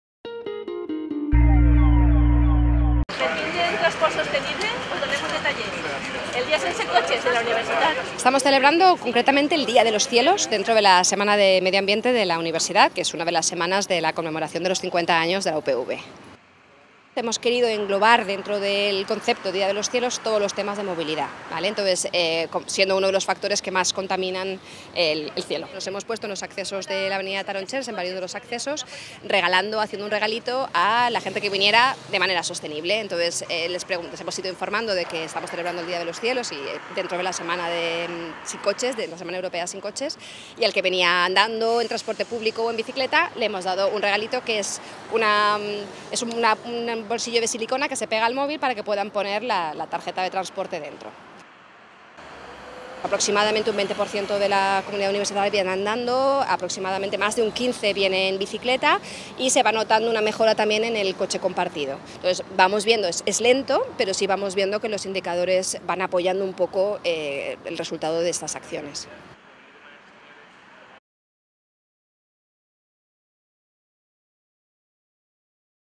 Día Sin Coches 2018 en la @UPV - Noticia @UPVTV, 20-09-2018
Dentro de la Semana del Medio Ambiente, uno de esos “5 días para cambiar el mundo” ha sido el Día Sin Coches. De forma indirecta, uno de los aspectos de mayor impacto ambiental de la actividad de la UPV es el ir y venir que generan las cerca de 40.000 personas que estudian y trabajan en esta universidad. Para ello, el Plan Estratégico de Movilidad Sostenible propone, anualmente, acciones progresivas para disminuir el transporte individual, en coche y moto, entre la comunidad universitaria.